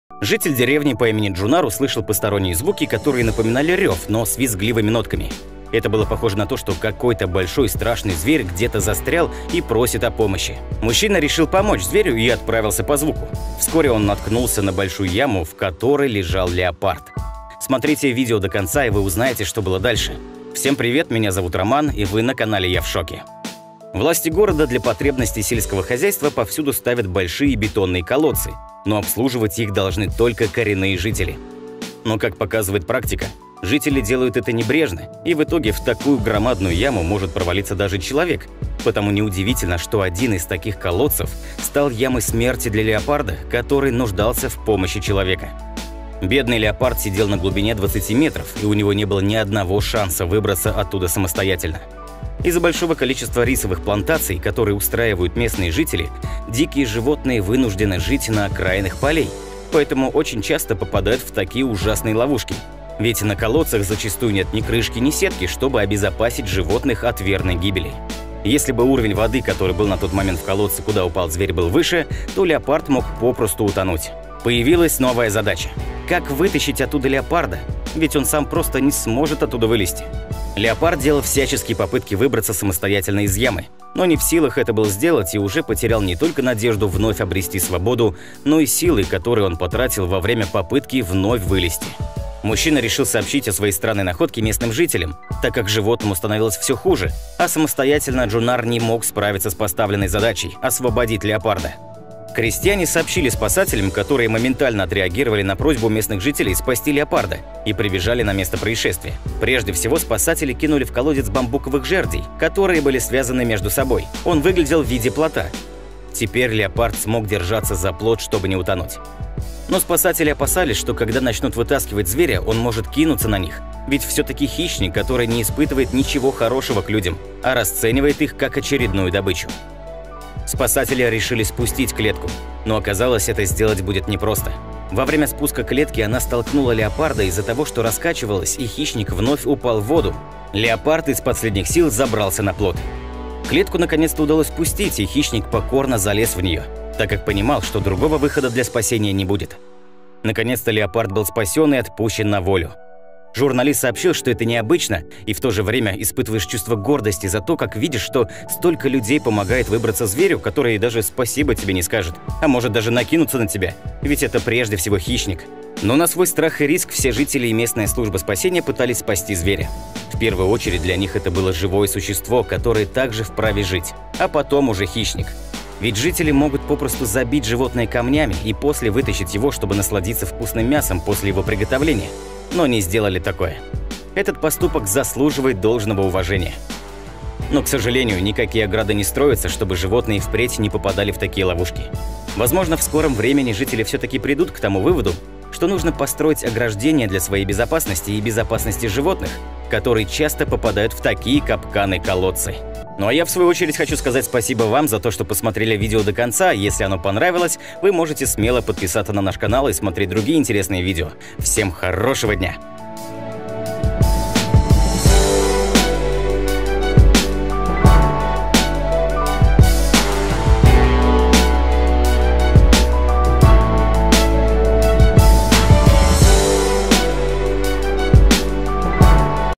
Мужчина услышал странный звук из колодца… То, что там оказалось, поразило всех!
Житель деревни по имени Джуннар услышал страшные звуки, которые были похожи на рёв, но с визгливыми нотками. Это было похоже на то, что какой-то страшный и большой дикий зверь где-то застрял и просит помощи.

Рекомендую посмотреть:
✓ Самые опасные и страшные острова в мире!
✓ Все гости были в восторге от платья невесты, но увидев ее лицо, застывали в ужасе!